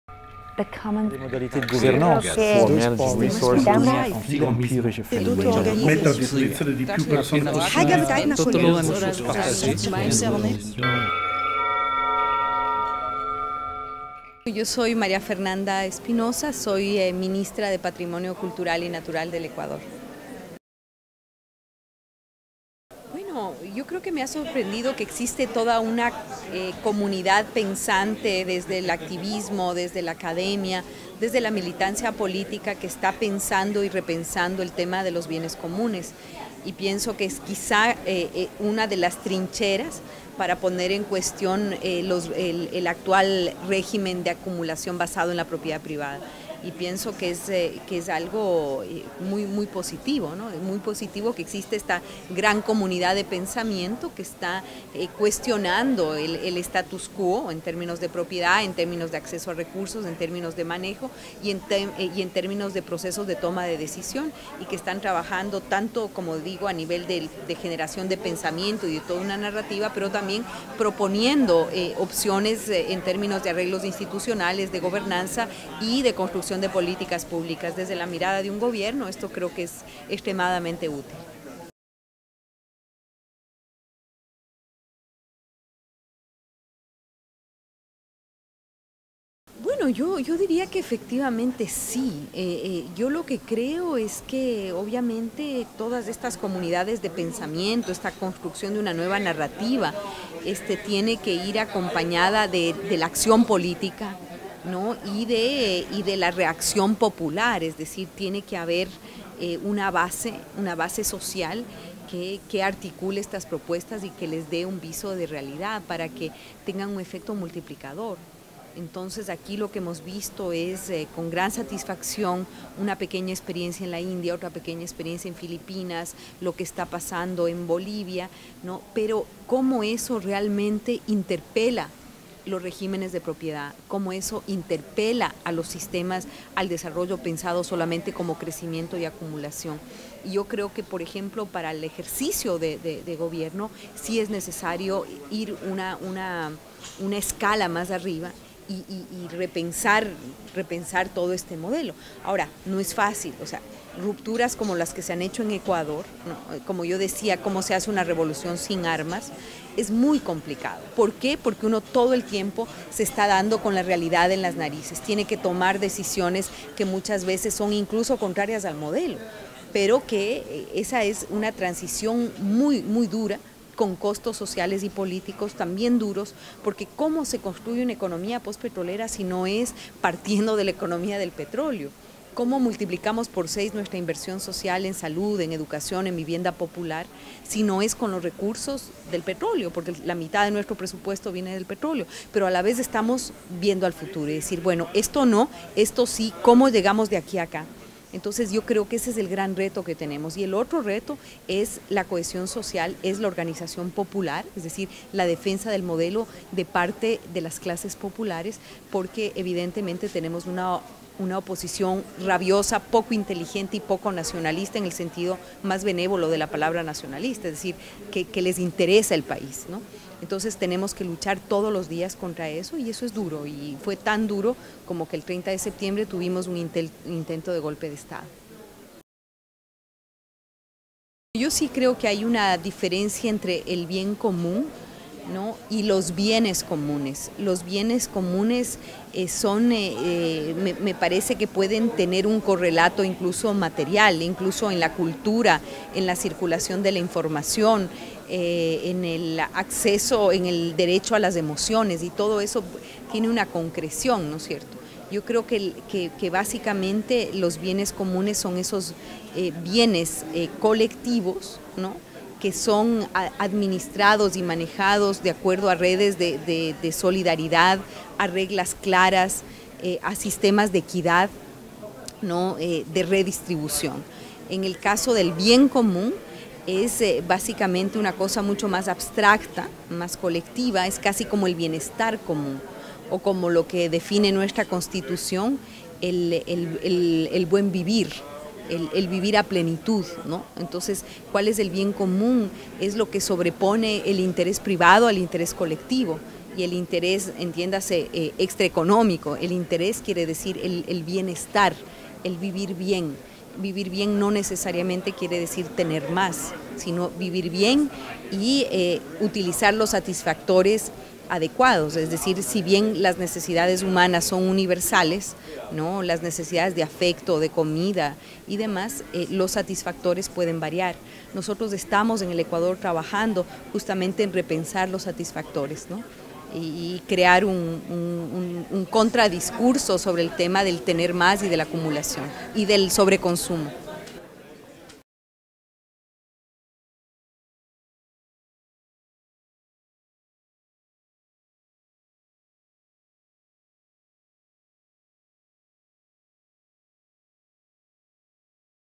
Pensar el Procomùn : Maria Fernanda Espinosa en Berlin
Entrevista realizada para el proyecto Remix the Commons en Berlin los 1-2 de noviembre 2010
durante la International Commons Conference

Entrevue réalisée pour le projet Remix the Commons à Berlin les 1-2 novembre 2010 lors de la Conference Internationale sur le Bien Commun

Interview produced for the Remix the Commons project at the Berlin International Commons Conference on Nov 1-2 2010.